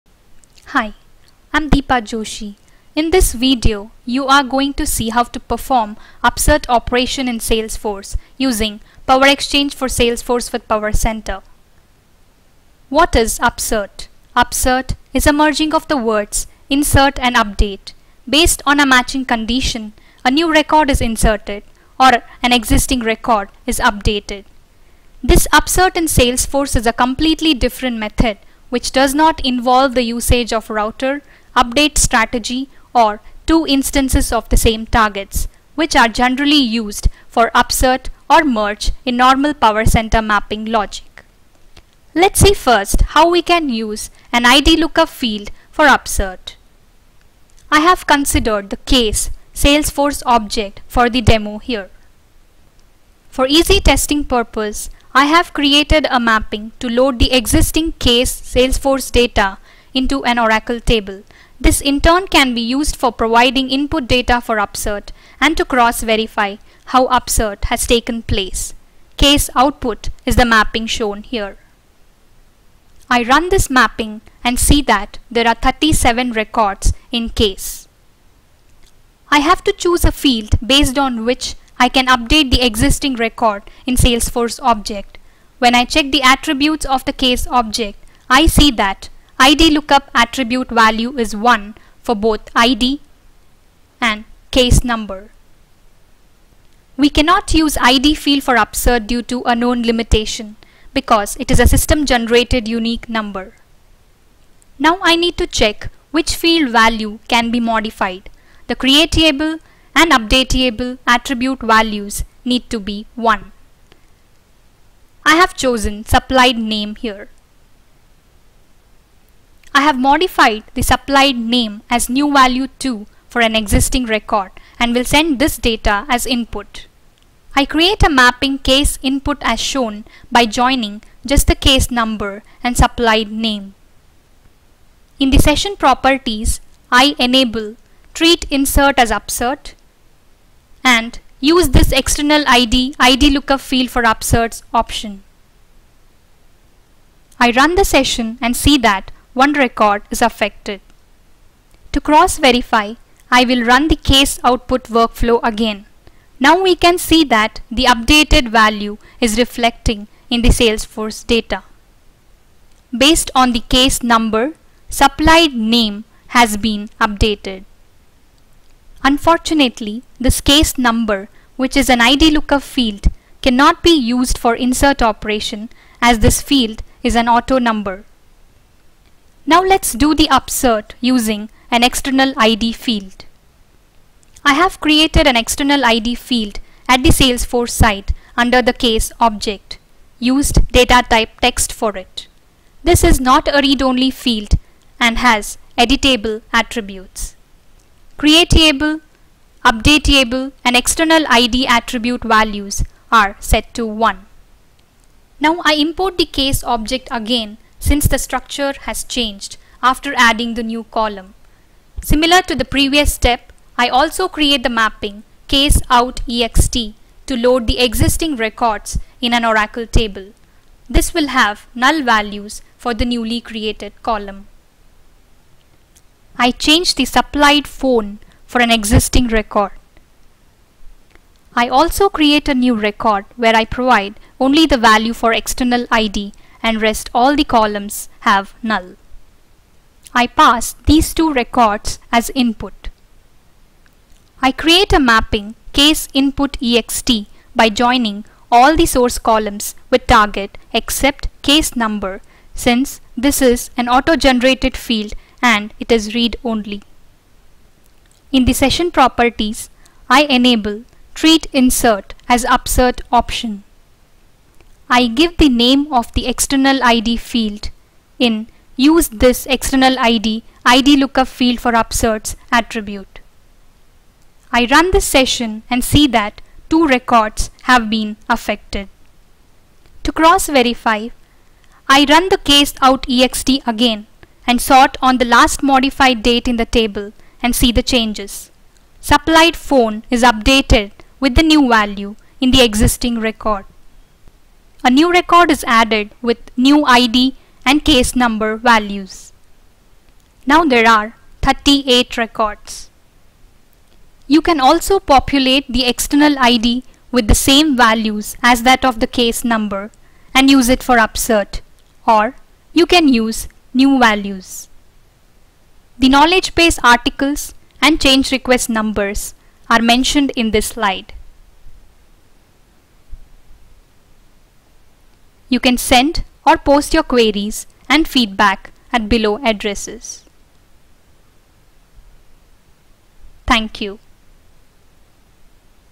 Upsert in Salesforce using PowerExchange for Salesforce with PowerCenter
In this video you learn about what is Upsert? How to perform upsert using IDLookup field and an External ID field in Salesforce.